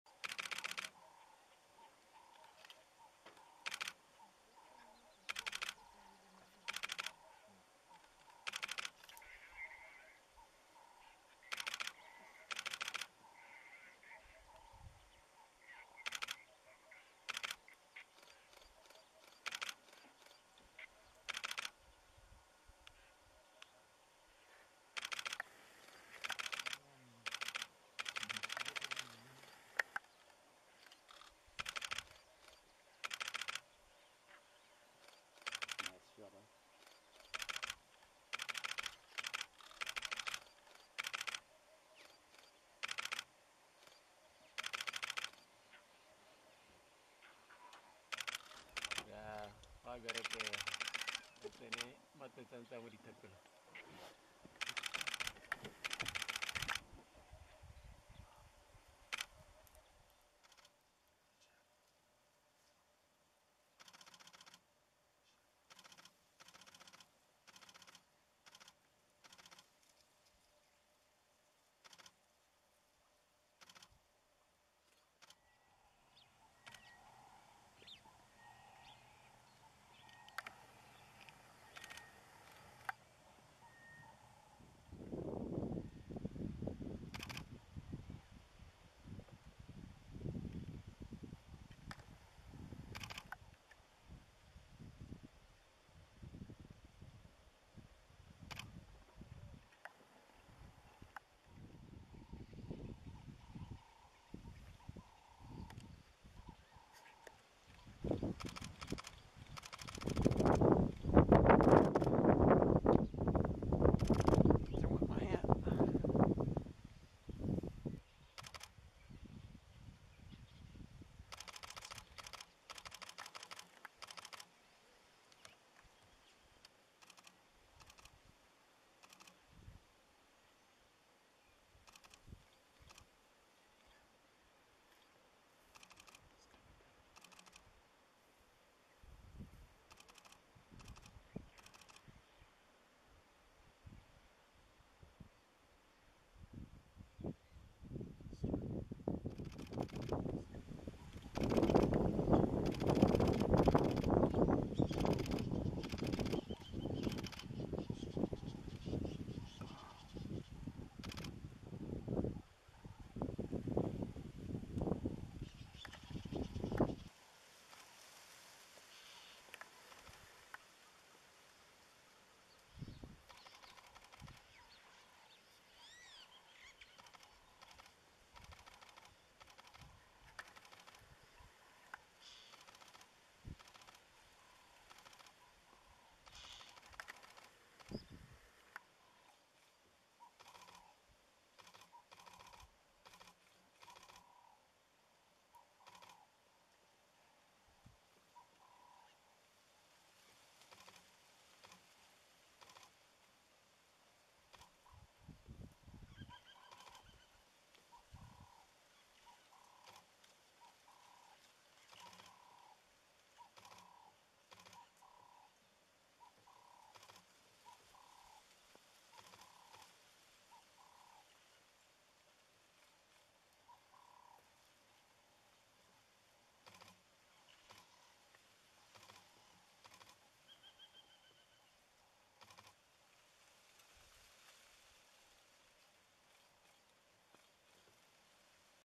The Leopards of Mashatu Botswana
We were lucky enough to have several leopard sightings at Mashatu in Botswana including (among others) two young leopards who tried to catch squirrels - one unsuccessfully and one successfully.